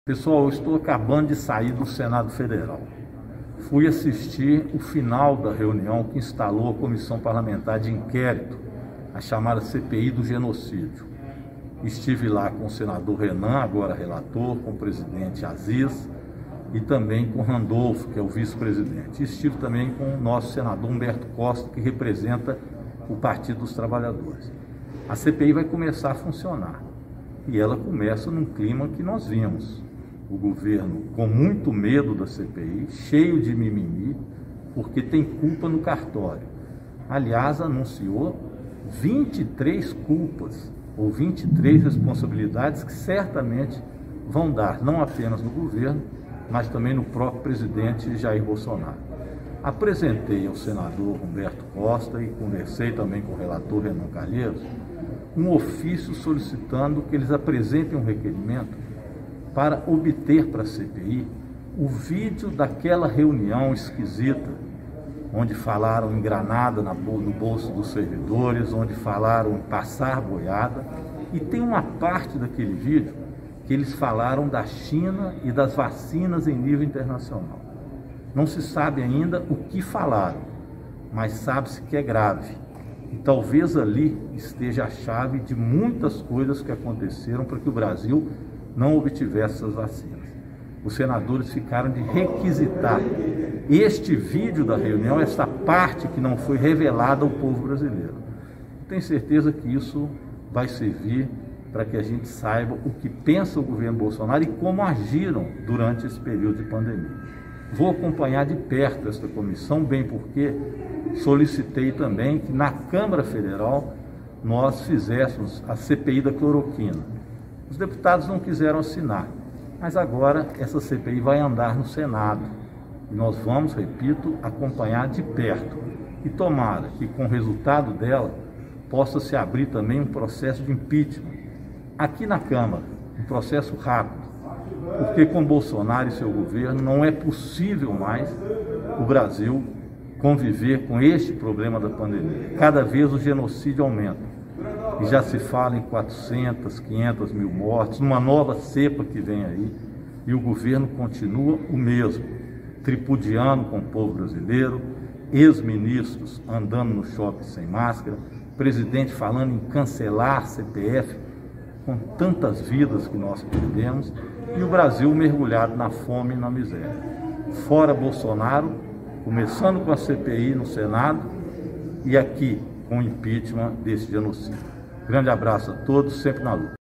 "Governo cheio de mimimi com a CPI"
Com uso de máscara e evitando aglomeração, o deputado federal Rogério Correia (PT-MG) foi a Brasília para acompanhar a abertura dos trabalhos da CPI da Pandemia, melhor conhecida como CPI do Genocídio. Ele encontrou-se com os líderes da CPI, protocolou requerimento e desejou boa sorte aos integrantes da comissão. “O governo Bolsonaro dá todos os indícios de culpa no cartório”, diz Rogério Correia. Confira neste vídeo de 3min.